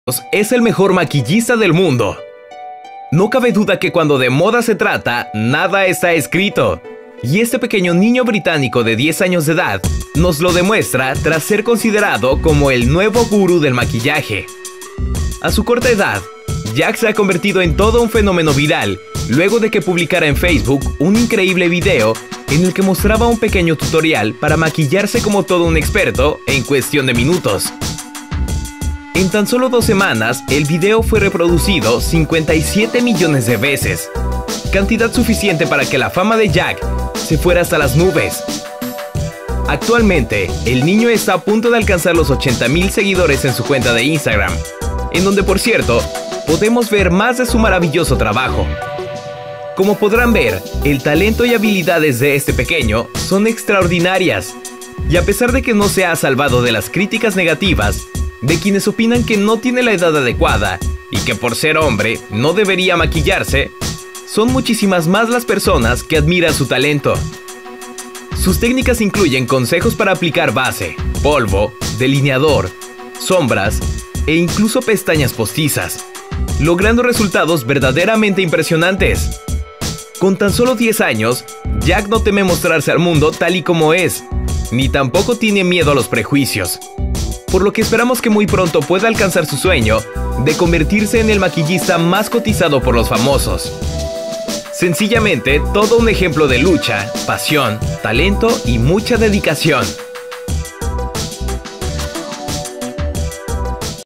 A sus 10 años es el mejor maquillista del mundo. No creerás cuánto gana por…
Dicen que en cuestiones de moda no hay nada escrito, y este niño británico de 10 años ha venido a comprobar que dicha afirmación es completamente cierta. Jack, a su corta edad, es llamado por los expertos ‘‘el nuevo gurú del maquillaje”.

Así es, leíste bien: “niño” y sí, tiene ¡10 años! Este pequeño se hizo famoso en sólo 2 semanas, a raíz de que uno de sus videos con tutoriales para maquillarse fue publicado en Facebook.

Dicho video ha sido visto más de 57 millones de veces, y fue suficiente para acelerar la fama de Jack de manera impresionante; está cerca de lograr 75 mil seguidores en su cuenta de Instagram.

Como pueden ver, sus habilidades son extraordinarias, y a pesar de que no se ha salvado de las críticas negativas de quienes opinan que no tiene ni el género ni la edad adecuada para maquillarse, son muchísimos más los que admiran su talento.

Su técnica incluye base, polvo, brochas, delineador, sombras e incluso pestañas postizas, y no tiene problemas para obtener resultados que muchas mujeres buscan al maquillarse.

BADABUN NETWORK ©